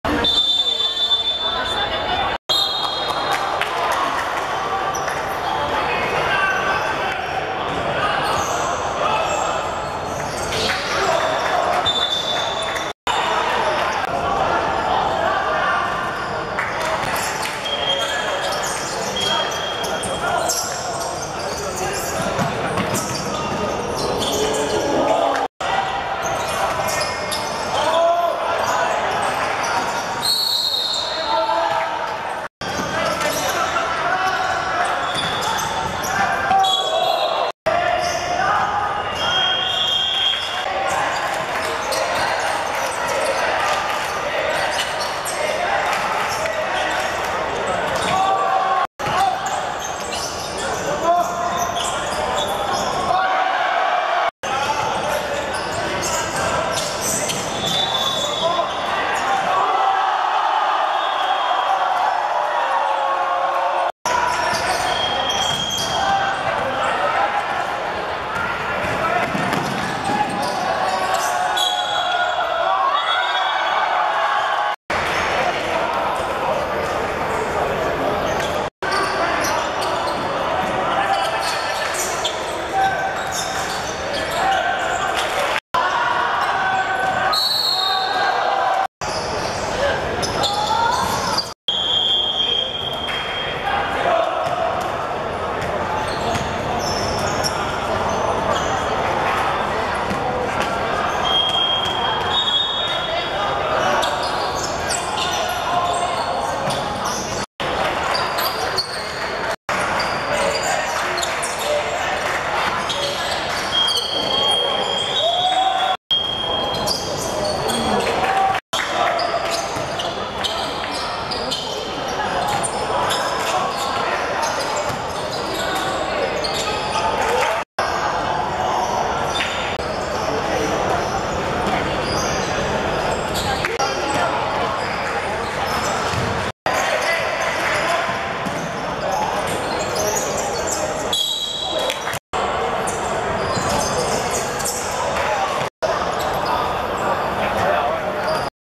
桂華山vs瑪利諾神父(2015.7.3.學界籃球馬拉松男子三十二強)精華
2015.7.3.港灣道體育館 16:45 桂華山(白) 30:9 瑪利諾神父(綠)